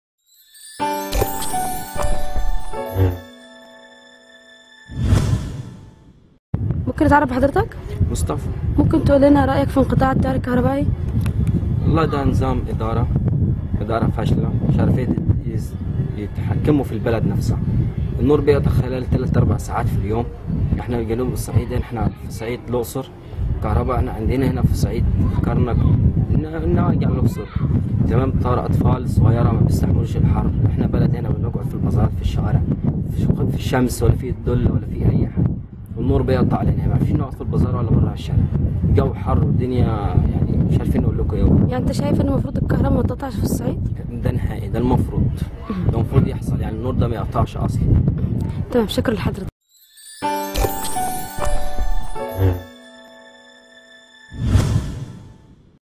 غضب بين أهالي الأقصر بسبب انقطاع الكهرباء
سيطرة حالة من الغضب على أهالي الأقصر، بسبب عودة انقطاع التيار الكهربائي، حيث إن حرارة الجو مرتفعة للغاية في المحافظة التي تقع في جنوب الصعيد لذلك فإن انقطاع التيار الكهربائي باستمرار يؤثر على حياتهم اليومية